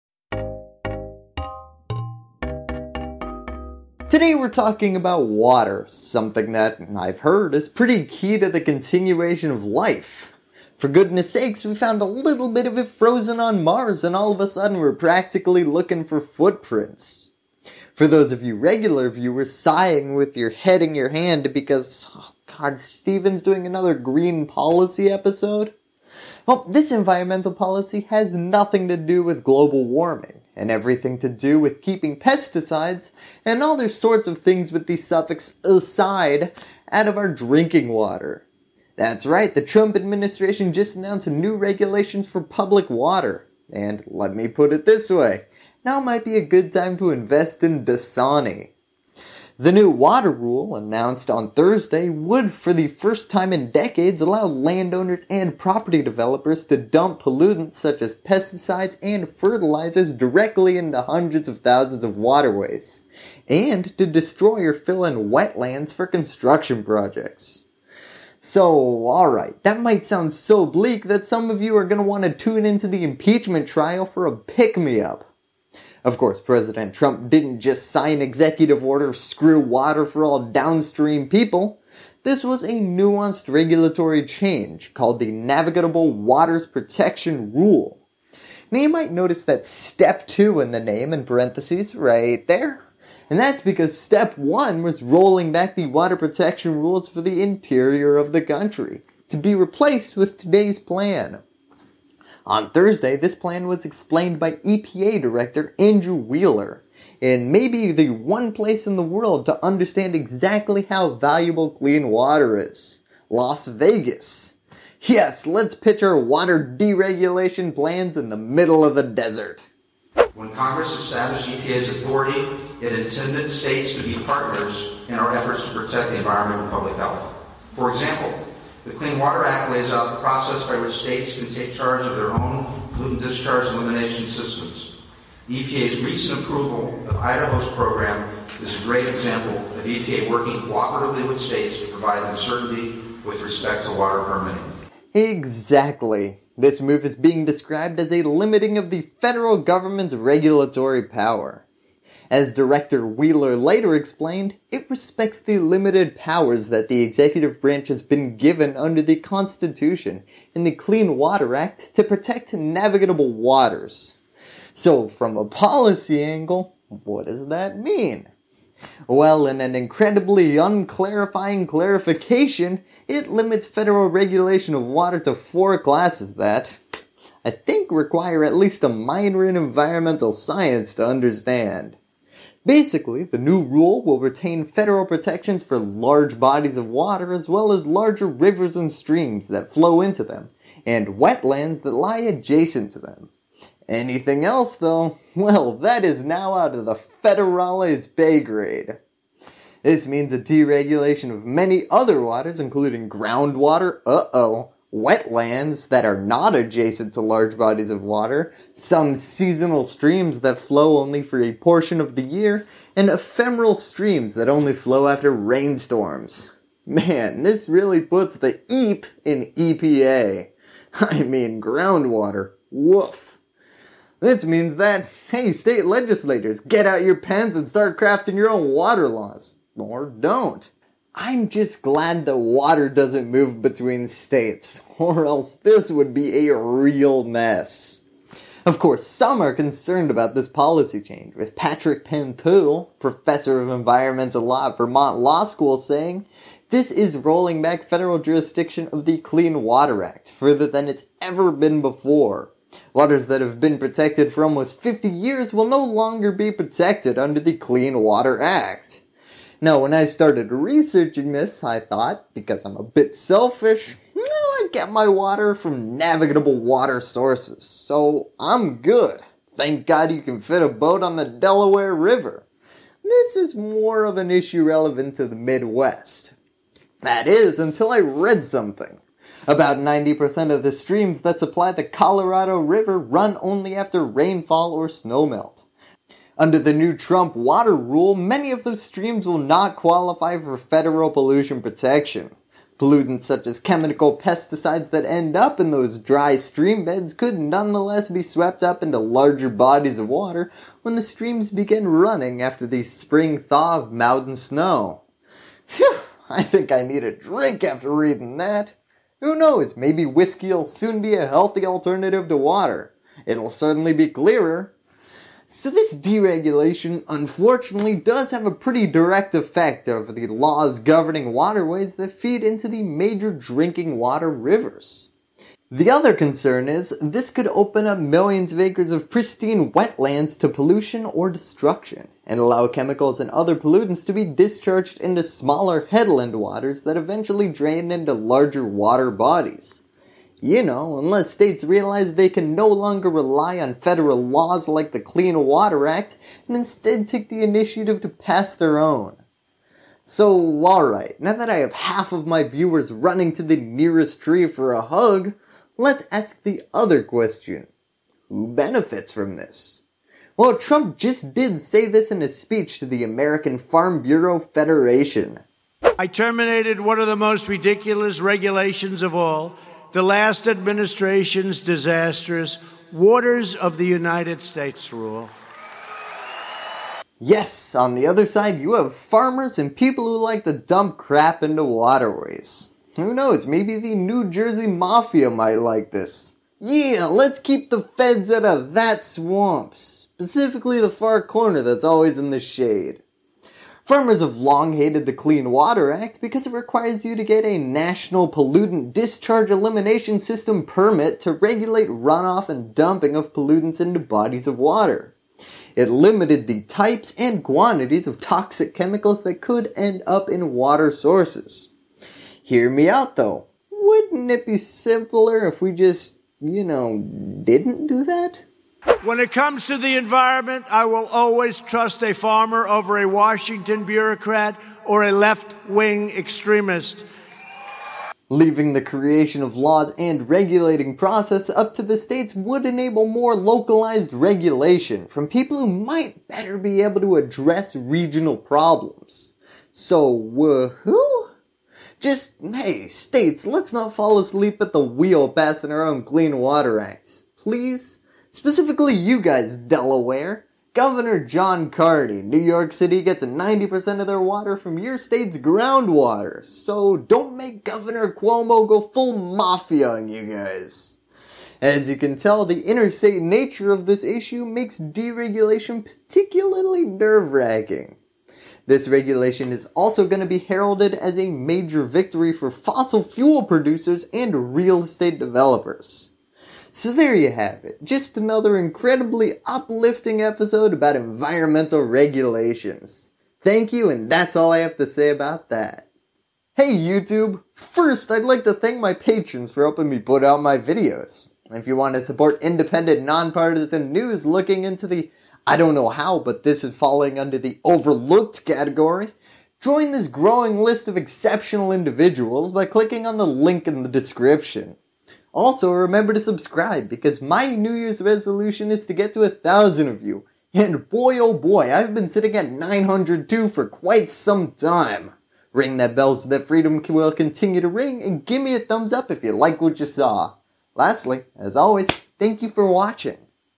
America’s New Drinking Problem? The Trump Administration Rolls Back the Clean Water Act
President Trump’s EPA just restricted the bodies of water that the federal government can regulate to: the territorial seas and traditional navigable waters, Perennial and intermittent tributaries to those waters, Certain lakes, ponds, and impoundments, and Wetlands adjacent to jurisdictional waters. This unfortunately means that the federal government is no longer enforcing laws over many other waters, including groundwater, wetlands that are not adjacent to large bodies of water, some seasonal streams that flow for only a portion of the year,  and “ephemeral” streams that only flow after rainstorms. Here’s what the means!